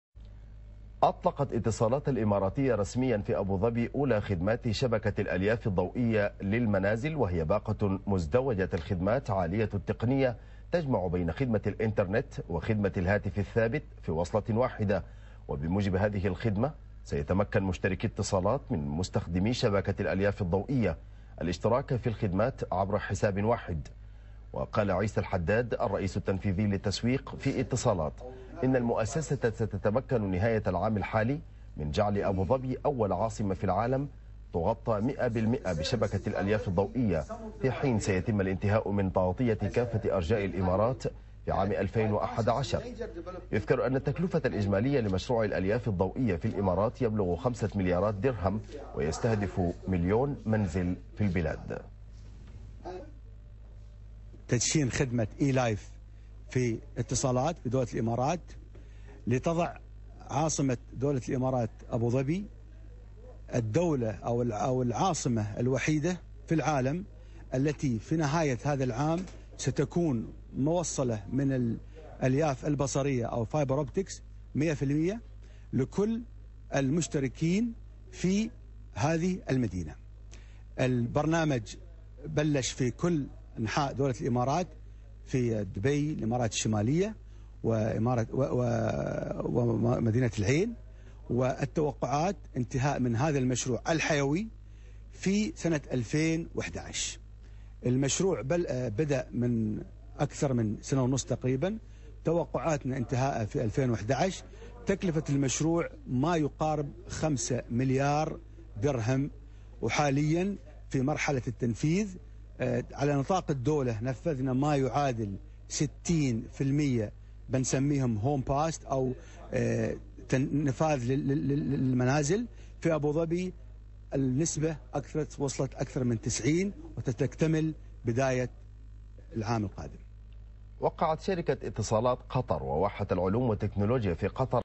Etisalat officially launches the 'eLife'
As part of the most anticipated launch of the year, Etisalat announced the roll-out of eLife which is set to introduce a whole new era of communication and entertainment in the UAE. As part of the coveted launch, Etisalat is offering the first-in-line double play bundles to its eLife (FTTH: Fiber to the Home) connected customers.eLife double play will offer benefits of convenience, coverage and value to customers by combining two of Etisalat's most popular services - fixed Broadband Internet and Landline services- in one bundled offer, delivered through a single access network.
CNBC Arabiya - 16/12/2009